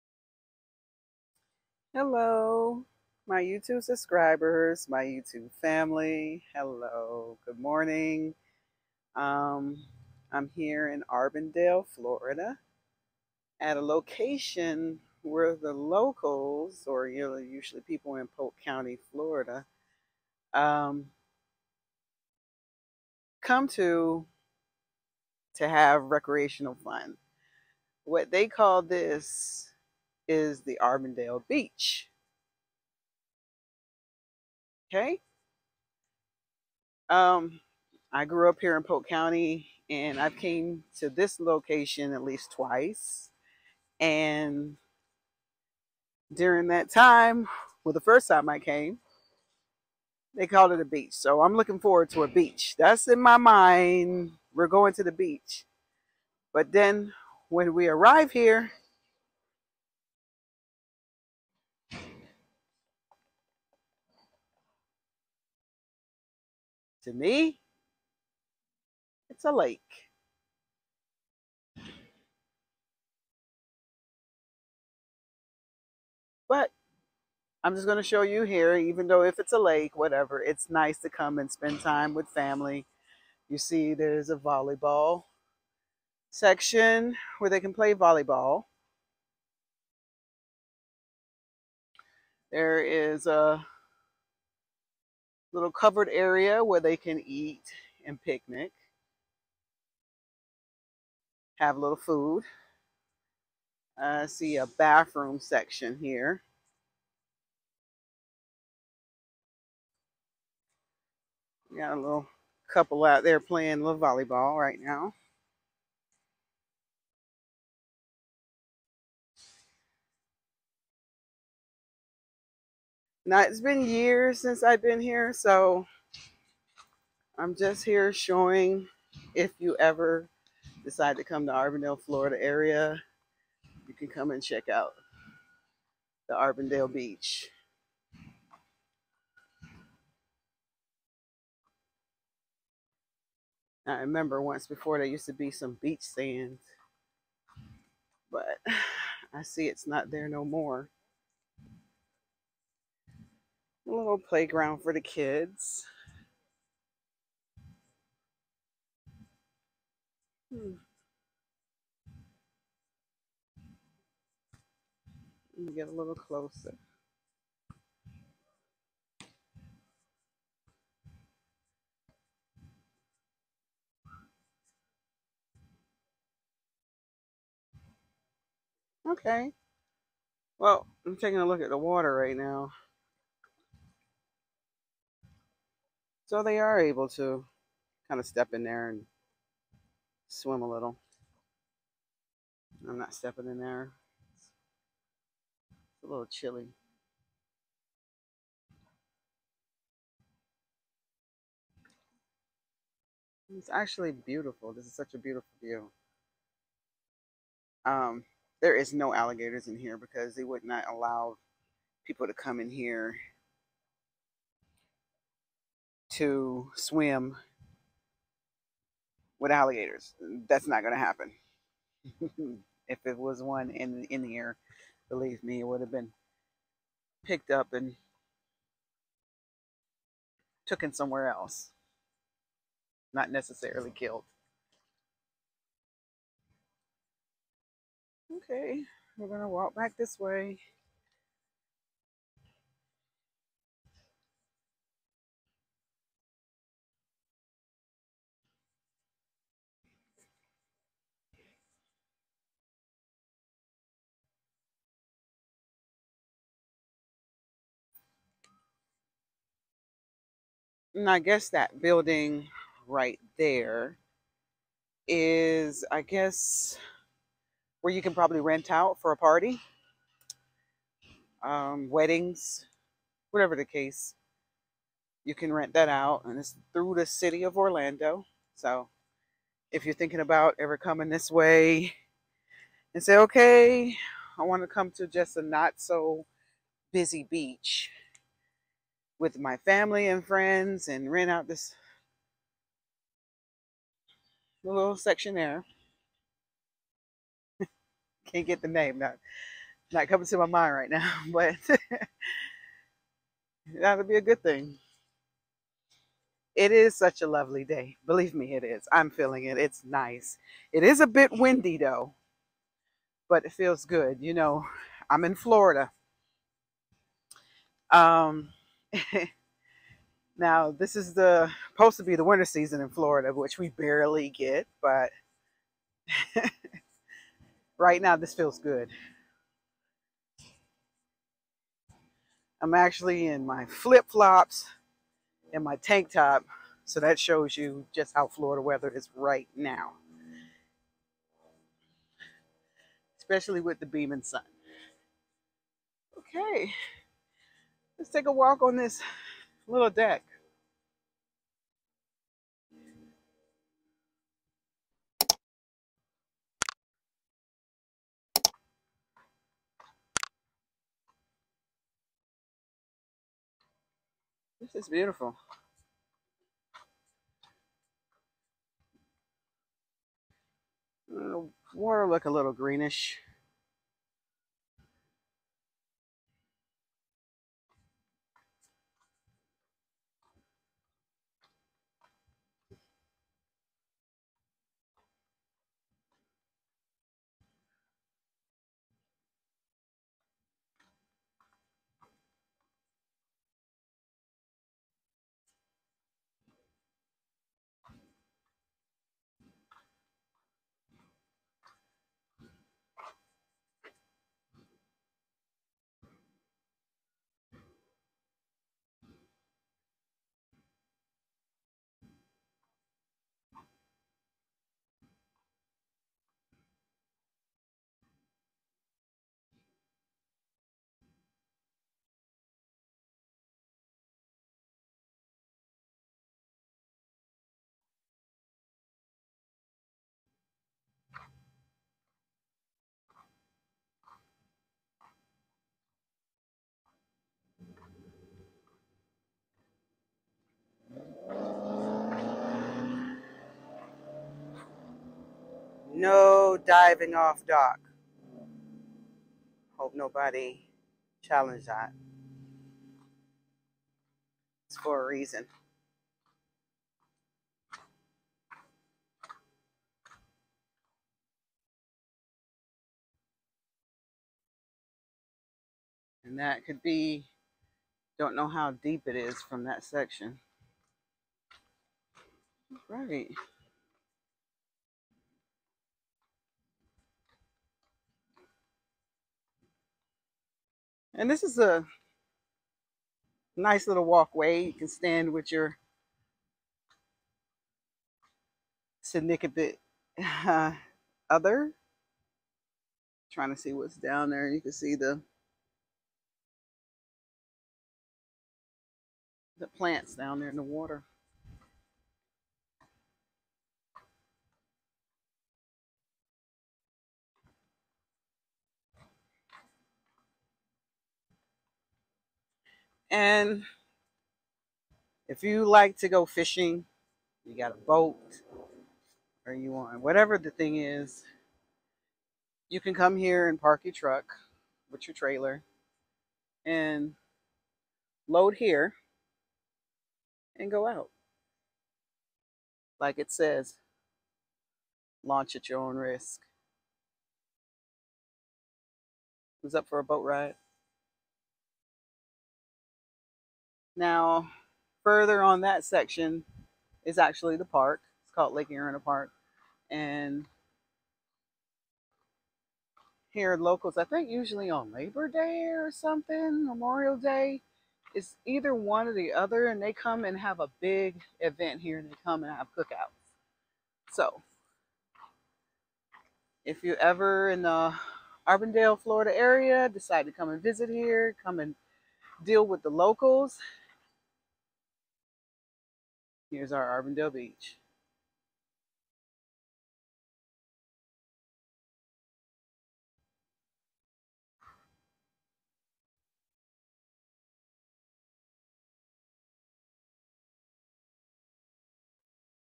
"Lake Ariana's Majestic Wonders: A Journey Through Auburndale, Florida"
Visiting  Lake Ariana at Auburndale, Florida the beach known by locals as the Auburndale beach.